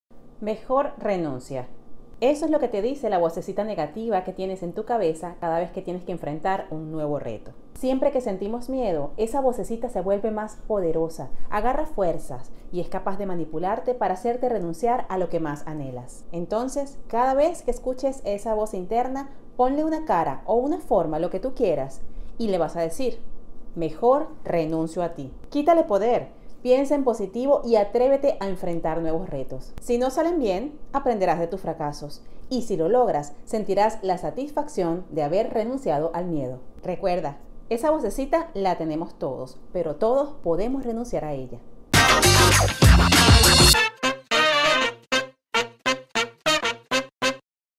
IsaTips - Renuncia
Hola mis oradores estrellas! Hoy les traigo un tip sobre cómo renunciar a nuestros miedos y a la vocesita negativa que tenemos en nuestra cabeza. Espero les sea útil!!!
Instagram y Tiktok: @IsabelPerozo
Cámara y Diseño Gráfico: Argenis López @lopezargenis
Edición: Isabel Perozo